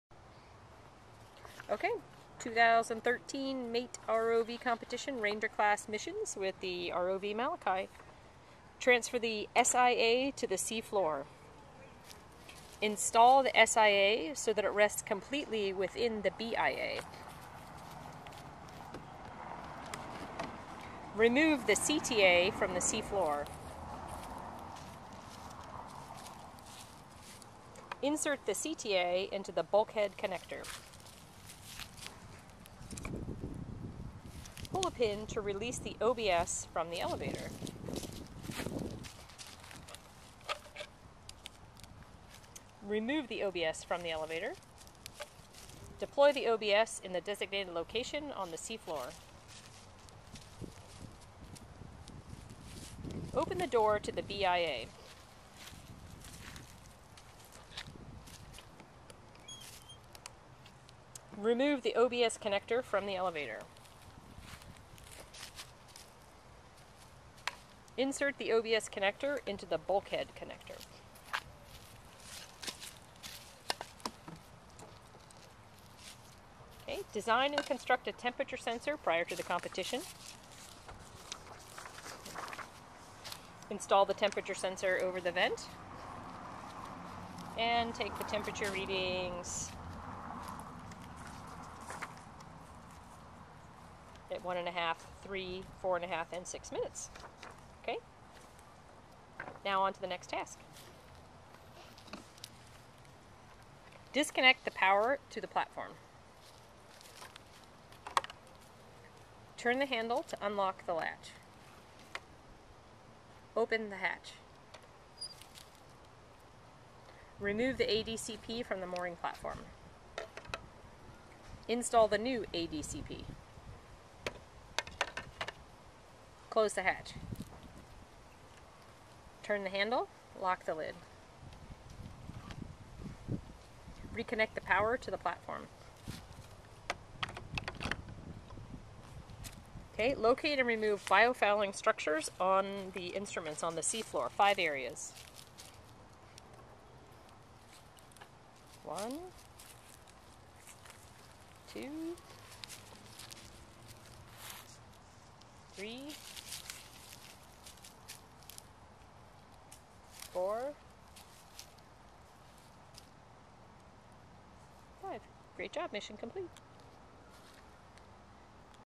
2013 Ranger Class Demonstration with ROV Malakai
ROV Malakai demonstrates the props and missions for the MATE 2013 Ranger Class.